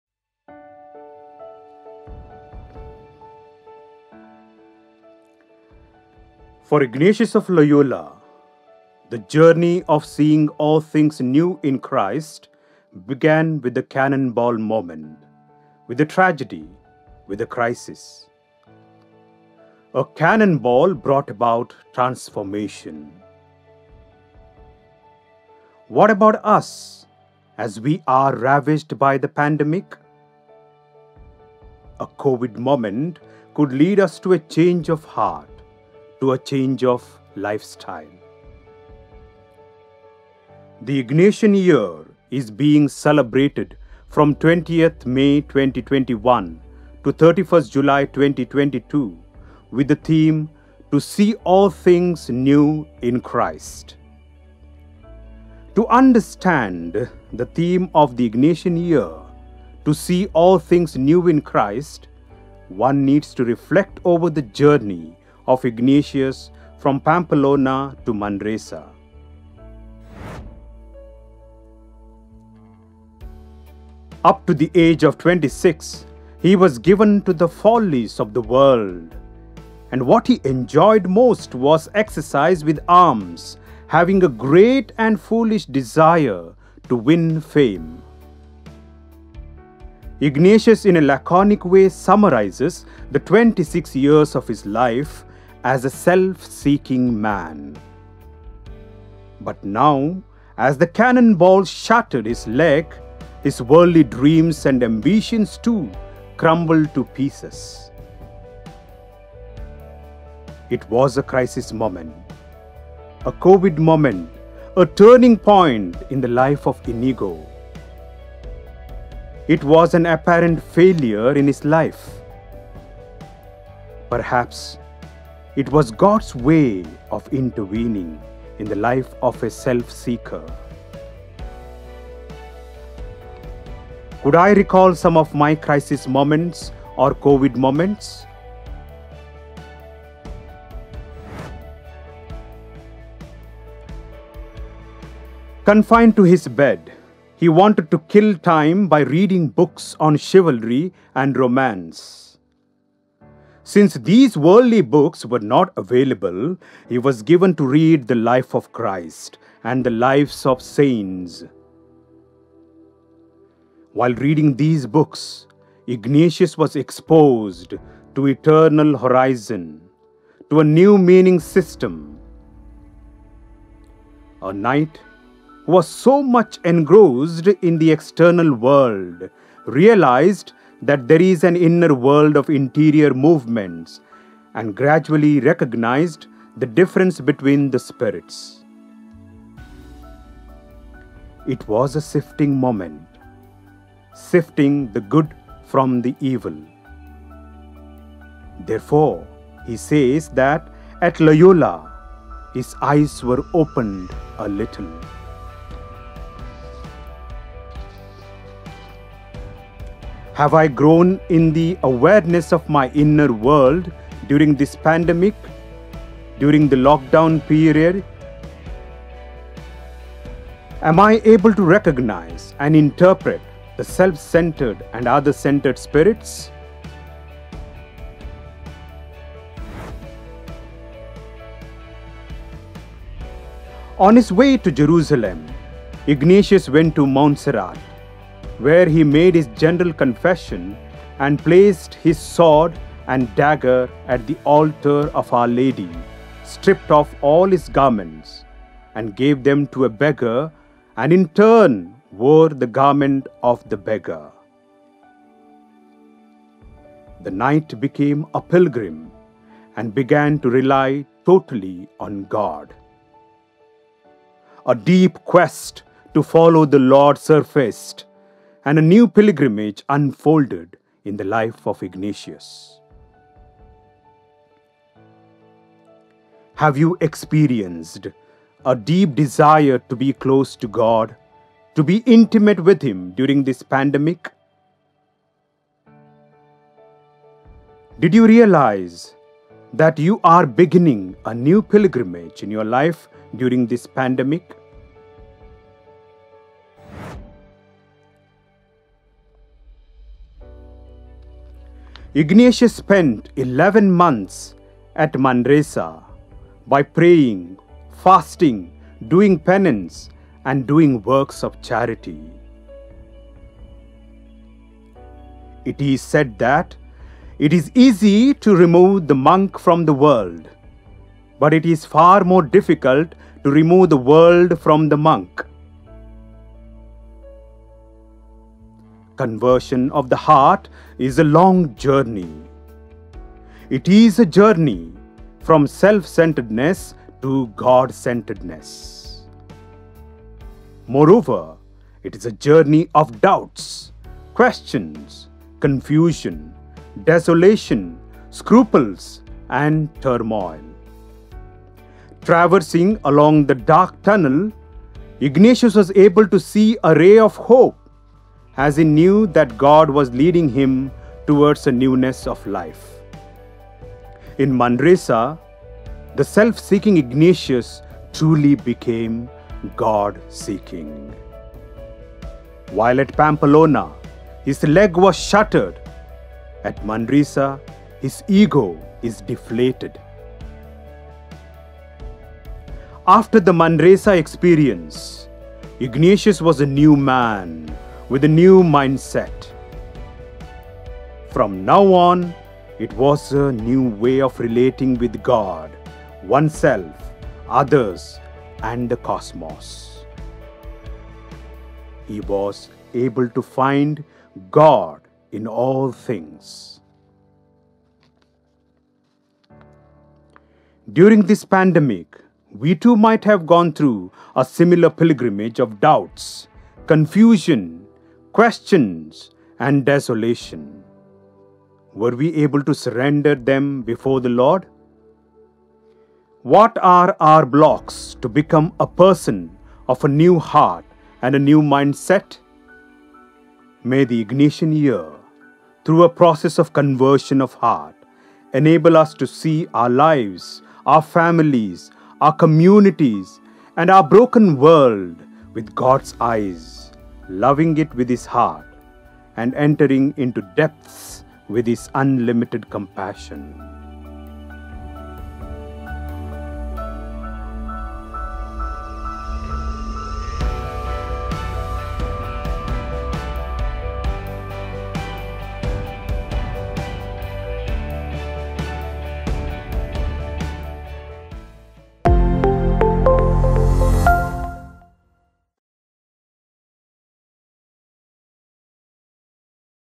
To see all things new in Christ...I
To see all things new in Christ is the theme of the Ignatian Year 2021. It's a journey of Ignatius from Pamplona to Manresa where he experienced the Unconditional love of God.

Video Credits:
Script: Jossie D'Mello SJ
Narration: Kiran Coth SJ
Editing: Vivian Richard SJ
Audio Recorded at XIM University, School of Communications

Motion Pictures from:
Stock VIdeo: Pexels
Audio: Audio Jungle